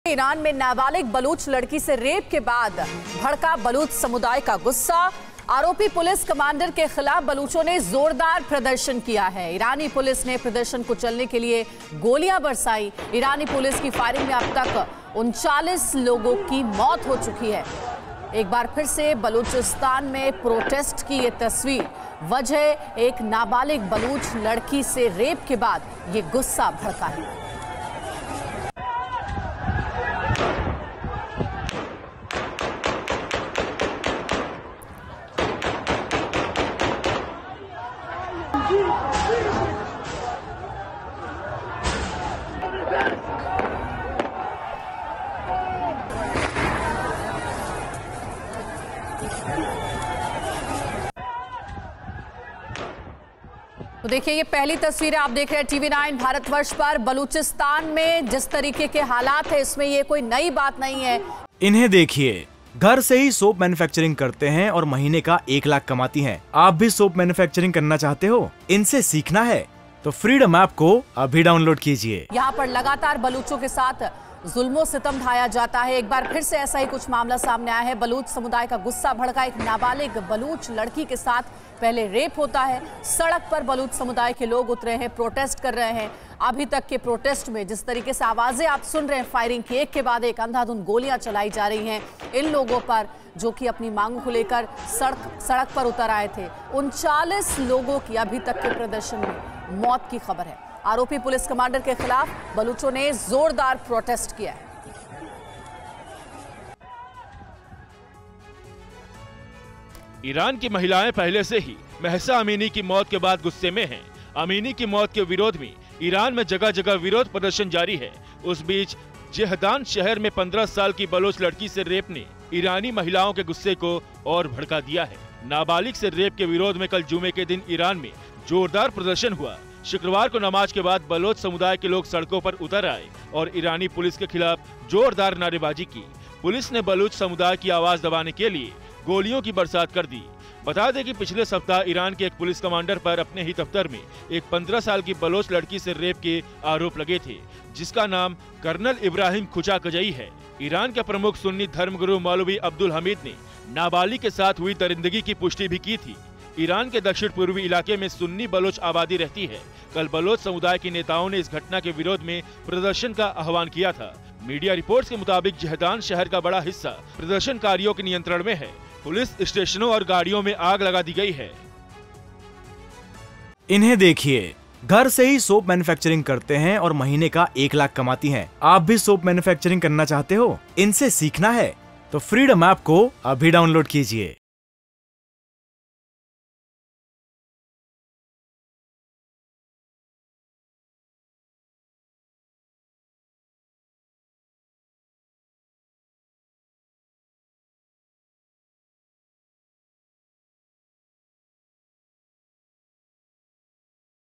Iran में नाबालिग Baloch लड़की से रेप के बाद जोरदार प्रदर्शन, Police ने बरसाई गोलियां
ईरानी महिला पहले से ही...Mahsa Amini की मौत के बाद गुस्से में है...आमिनी की मौत के विरोध में...Iran में जगह-जगह विरोध प्रदर्शन जारी है...उस बीच जेहदान शहर में 15 साल की बलोच लड़की से रेप ने ईरानी महिलाओं के गुस्से को और भड़का दिया है ।